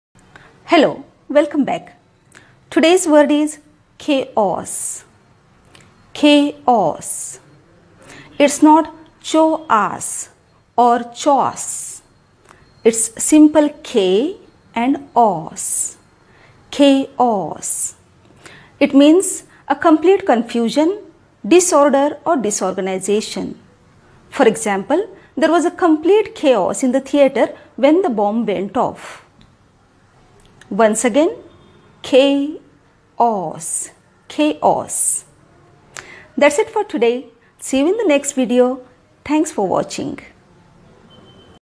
chaos meaning/English word meaning/daily use words/dailyuse vocabularywords/englishwithshivkanya
Hello!
I am Shivkanya, your English teacher.
You will learn the proper pronunciation of the word 'chaos'.